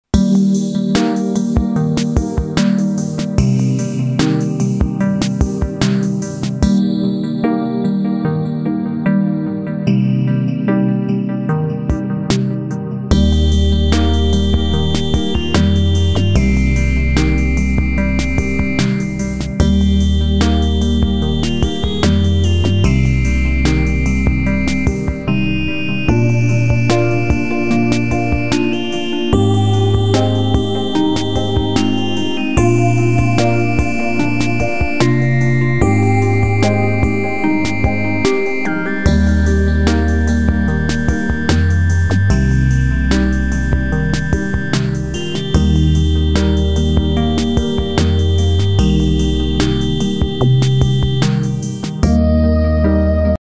Rosy Wolf Snail (music was only added to omit audio)
Rosy Wolf Snail is a predator species that must eat other snails to live. This one was found in the wild to make a video of, then released. He had a damaged shell from the wild, not me.